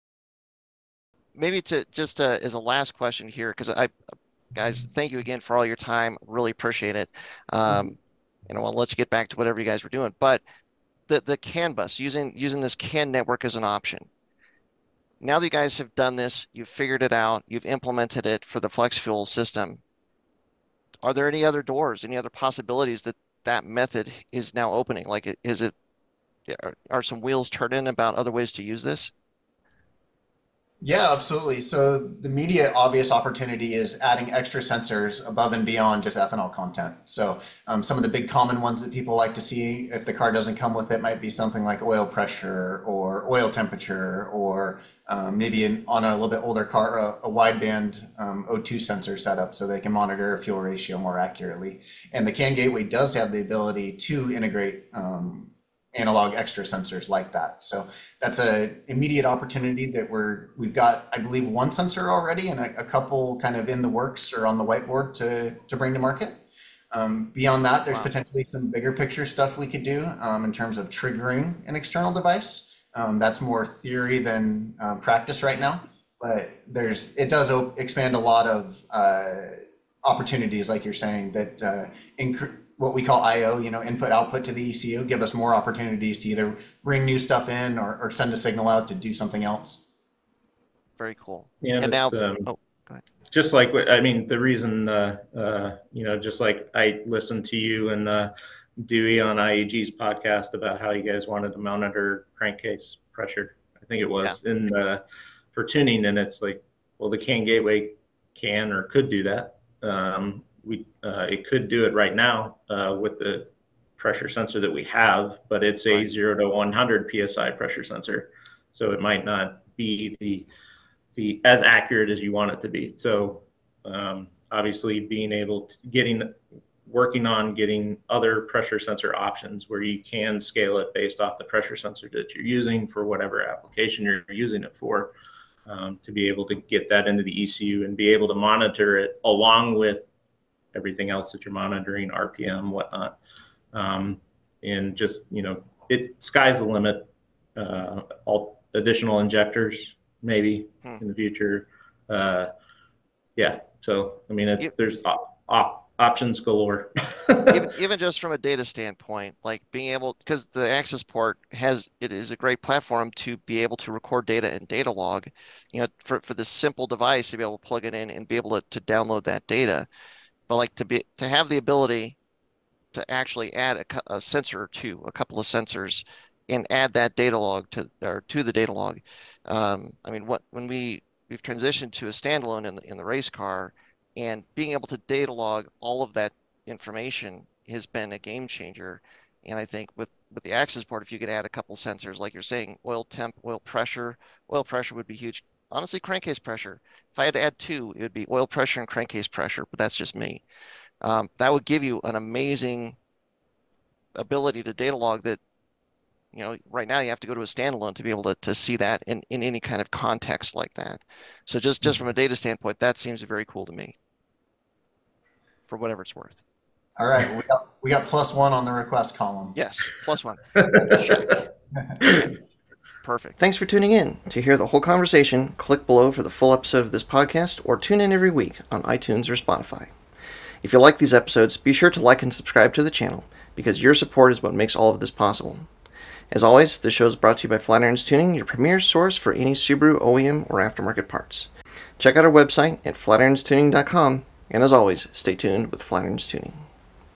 Can Gateway has lots of Potential!
At the end of Podcast episode #145 with Lance and Billy from Cobb Tuning, I asked about the CAN Gateway that they developed to make Flex Fuel possible on the newer direct-injected WRX.

They use this to add signals to the cars factory CAN Bus, and it seemed like there may be a lot of potential there.

We talk about what this new device may be able to unlock in terms of function and Datalogging in the future.